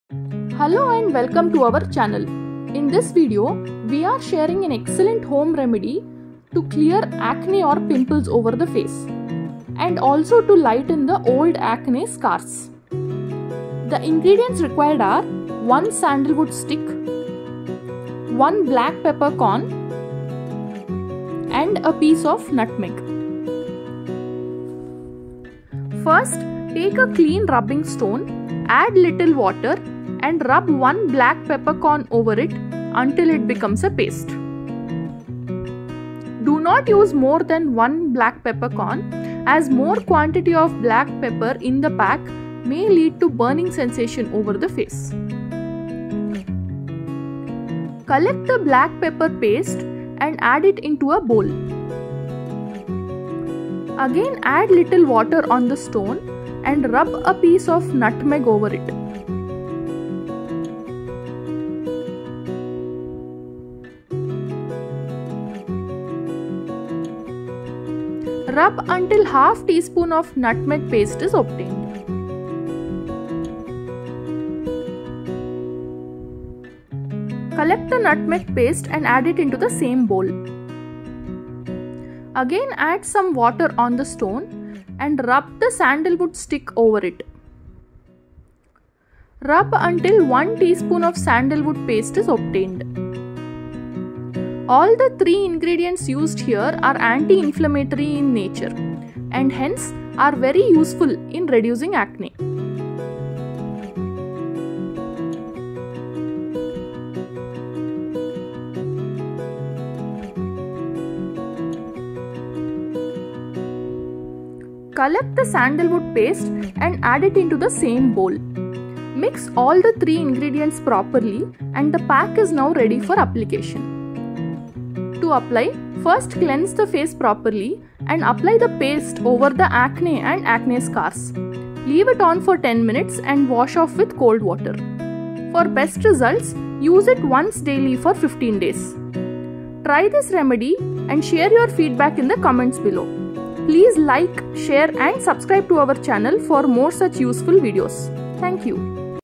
Secret Home Solution for Acne and Scars
In this video we are sharing an excellent home remedy for reducing acne or pimples and also to lighten the acne scars.
This is very simple to make and use. It can be prepared with easily available ingredients.
Ingredients:-
One sandalwood stick (గంధపు చెక్క)
One black pepper corn (ఒక మిరియం గింజ)
 Note:- Do not use more than one black pepper corn as more quantity of black pepper in the pack may lead to burning sensation over the face.
A piece of nutmeg (ఒక జాజికాయ ముక్క)

Preparation:-
Take a clean rubbing stone and add little water over it. Then rub one black pepper corn over it until it becomes a paste.
Collect the black pepper paste into a Bowl.
Then add little more water on the stone and rub a piece of nutmeg over it until 1/2 tsp of nutmeg paste is obtained. Collect the nutmeg paste into the same bowl.
Similarly, add little more water on the stone and rub the sandalwood stick over it until 1 tsp of the sandalwood paste is obtained.
Now mix paste of all the three ingredients properly and the pack is ready for application.
This pack must be prepared and applied immediately. it cannot be stored

Usage:-
First cleanse the face properly and apply this paste over the active acne and also the old acne scars, leave it on for 10 minutes and then wash off with cold water.
For best results, use it once daily for 15 days.

ಮೊಡವೆ ಮತ್ತು ಅದರ ಕಲೆಗೆ :
ಮಾಡುವ ವಿಧಾನ
ಒಂದು ಗಂಧ ತೆಯ್ಯುವ ಕಲ್ಲನ್ನು ಸ್ವಚ್ಛವಾಗಿ ತೊಳೆದುಕೊಳ್ಳಿ, ನಂತರ ಒಂದು ಚಮಚ ನೀರನ್ನು ಹಾಕಿ ಒಂದು ಕರಿಮೆಣಸಿನ ಕಾಳನ್ನು ತೆಯ್ದು ಅದರ

Like our videos and share it with your family and friends if you find it useful.